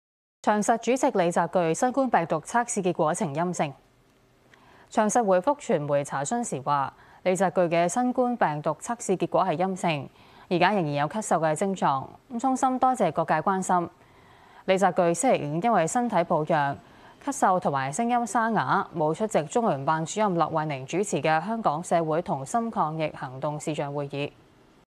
李澤鉅新冠病毒測試結果呈陰性 目前仍有咳嗽症狀
長實主席李澤鉅新冠病毒測試結果呈陰性。

長實回覆傳媒查詢時指，李澤鉅的新冠病毒測試結果是陰性，現時仍有咳嗽症狀，衷心多謝各界關心。

李澤鉅周五因身體抱恙，咳嗽及聲音沙啞，無出席中聯辦主任駱惠寧主持的香港社會同心抗疫行動視像會議。